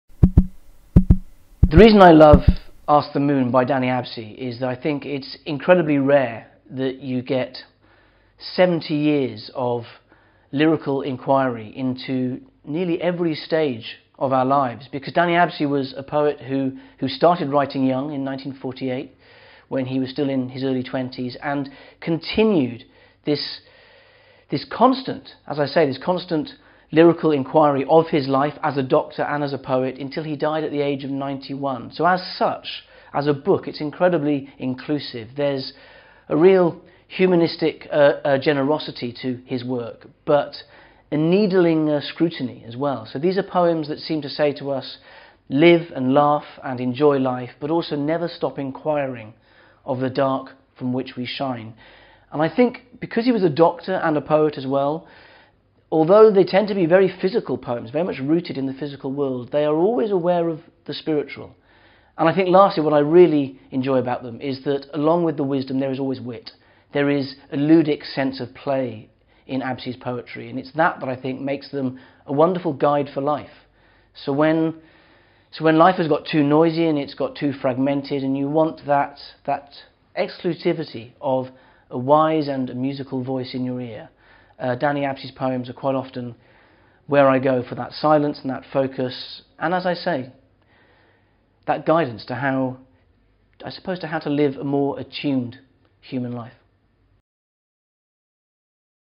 Owen Sheers on Ask the moon - Dannie Abse
– BOEKENTIPS IN 1 MINUUT – "I think that it's incredibly rare that you get seventy years of lyrical enquiry into nearly every stage of our lives." – Owen Sheers, 41, schrijver, dichter, Talgarth (Wales).